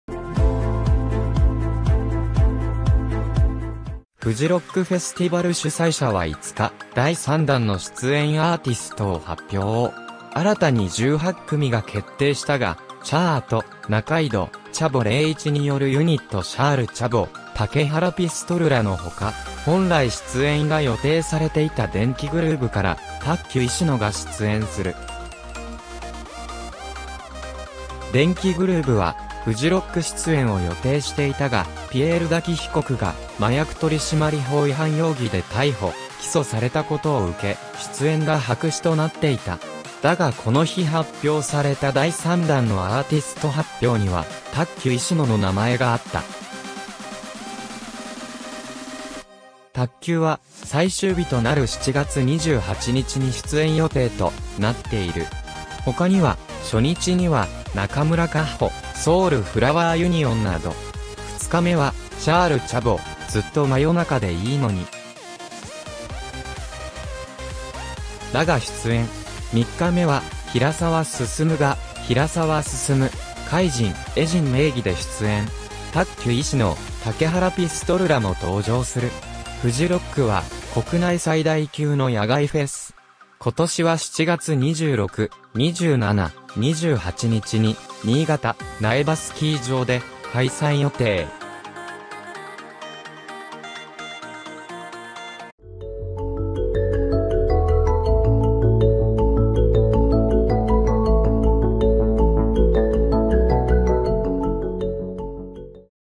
フジロック、石野卓球が出演 電気グルーヴは出演中止もソロで
フジロックフェスティバル主催者は５日、第三弾の出演アーティストを発表。新たに１８組が決定したが、ＣＨＡＲと仲井戸“ＣＨＡＢＯ”麗一によるユニットＣｈａｒ×Ｃｈａｂｏ、竹原ピストルらの他、本来出演が予定されていた電気グルーヴから、ＴＡＫＫＹＵ　ＩＳＨＩＮＯが出演する。
　電気グルーヴはフジロック出演を予定していたが、ピエール瀧被告が麻薬取締法違反容疑で逮捕、起訴されたことを受け、出演が白紙となっていた。だがこの日発表された第３弾のアーティスト発表にはＴＡＫＫＹＵ　ＩＳＨＩＮＯの名前があった。卓球は